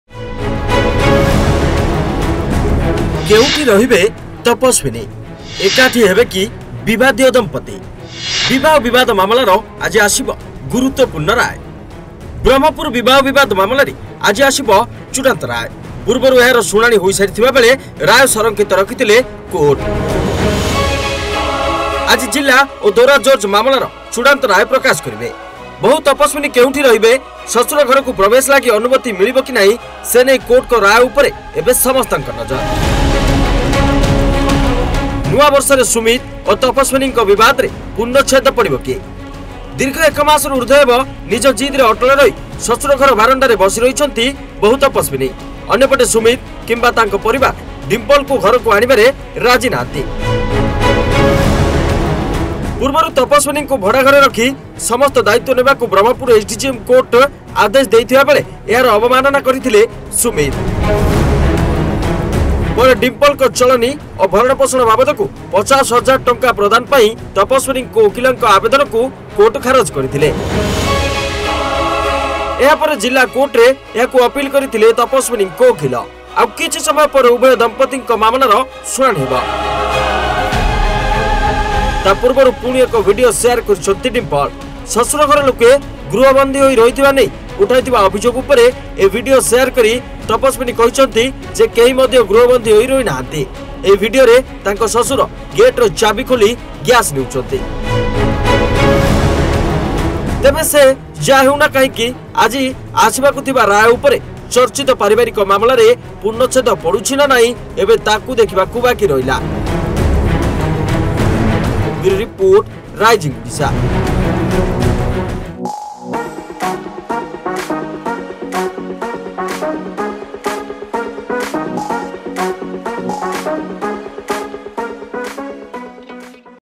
Tapaswini-Sumit Marital Dispute : Will Tapaswini & Sumit Unite ? | ଏକାଠି ହେବେ ସୁମିତ୍‌-ଡିମ୍ପଲ୍‌ !
The final verdict in the Berhampur marriage dispute case will come today.